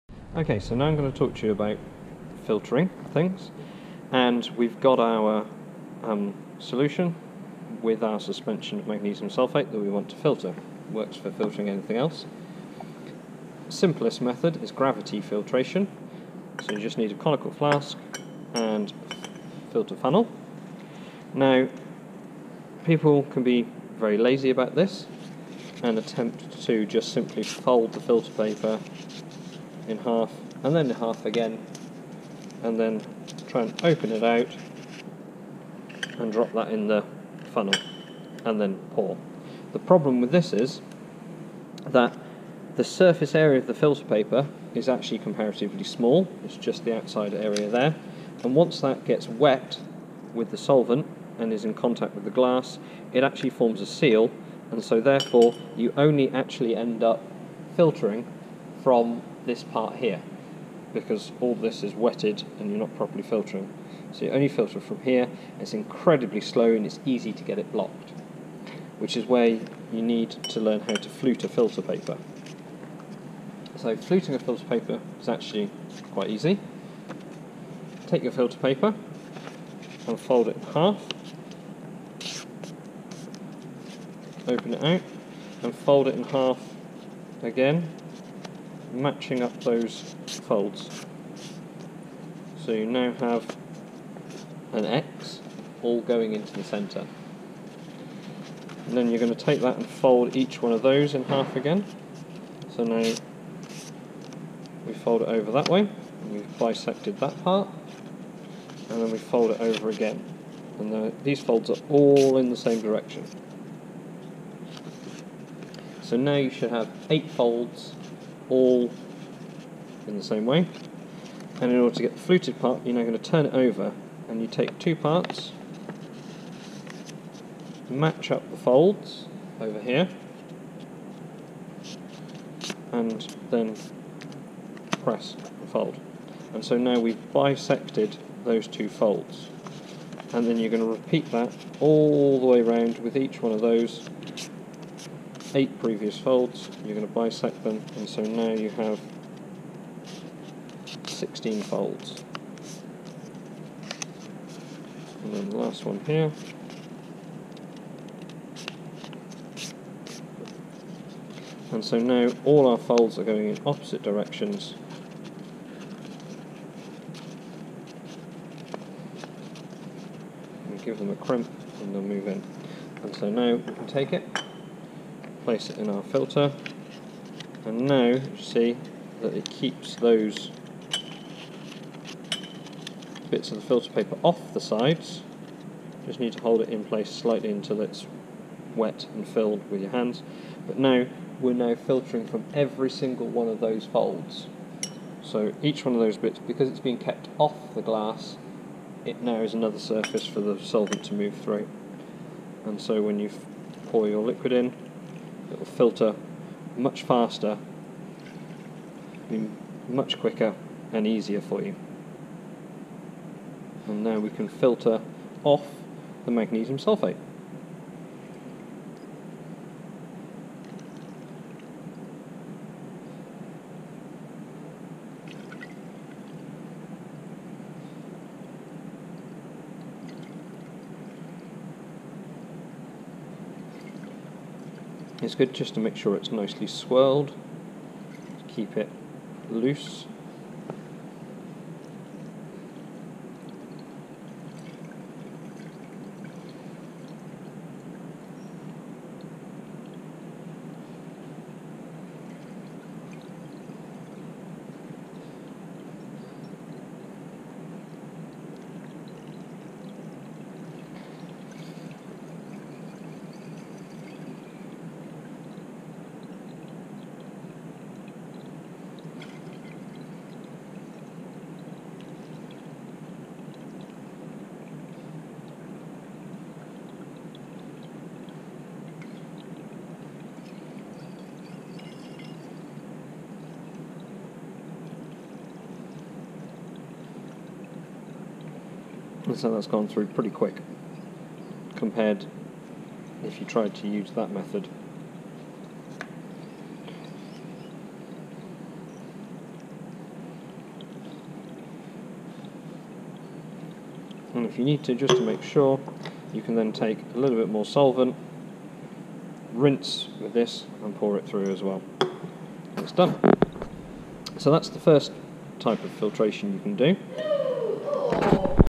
Gravity Filtration
Dr Stephen Flower from the Department of Chemistry demonstrates gravity filtration